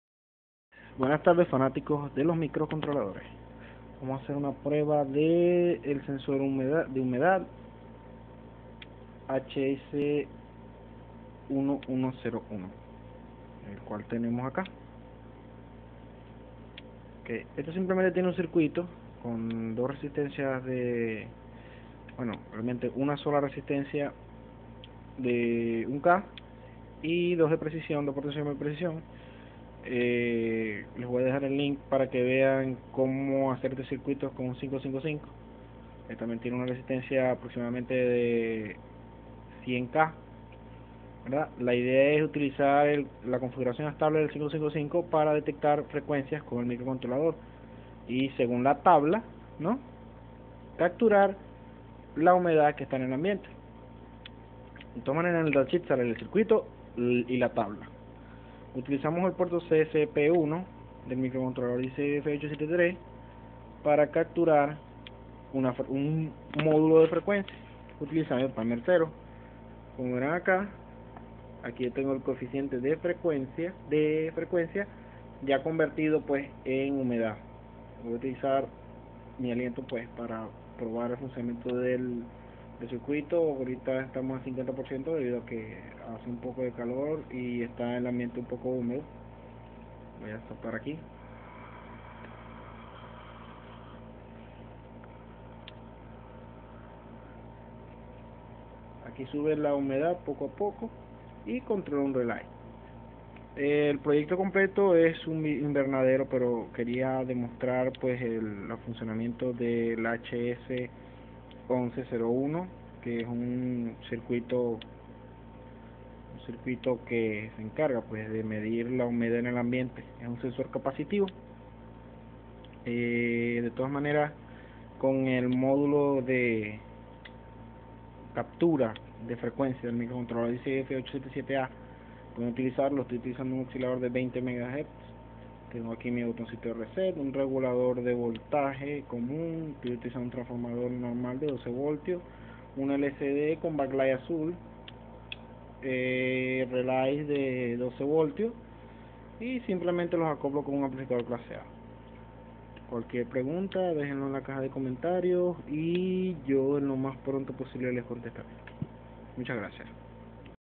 Microelectronica Venezolana Sensor HS1101
Procesador de 8 bits, 20Mhz, 16F877A, Lcd 16x2, Sensor HS1101

Pagina 3

Caracteristicas UeV ALE PRO:
Color Verde, Rojo y azul
Almentacion de 8-24Voltios AC/DC
Reguador de 5V integrado con disipador
Indicador de voltaje inverso
Salida/entrada de 5v/Voltaje de alimentacion en DC
Boton MCLR integrado u en bornera
Oscillador de 4Mhz o 20Mhz (Preguntar)
Pic Soportados 16F y 18F (Preguntar por aplicaciones especiales)
Lcd 16x1/16x2/16x4/20x1/20x2/20x4 BackLigth Verde, Roja, aZUl
Inputs 6 Analogas/digitales 0/5V
Input Captador de frecuencia 5Vmax
Outputs integradas 4 SPDTRelay Con Led indicador (Posible expansion +4)
Ouyput PWM
Conector listo para usar REAL TIME CLOCK(Modulo no incluido)
Buzzer y led indicador incluido
Conector para salida serial 232 (Con Sofware incluido bajo pedido)